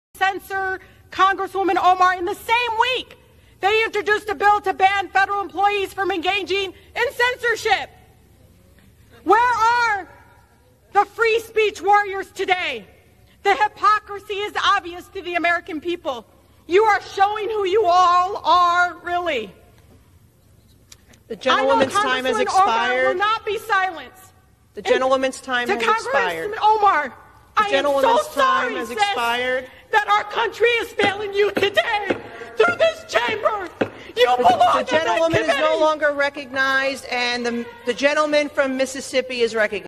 Rep Rashida Tlaib GOES BERSERK Over Antisemite Omar's Possible Removal From Committee
Rep Rashida Tlaib: "I'm so sorry that this country is failing you today through this chamber."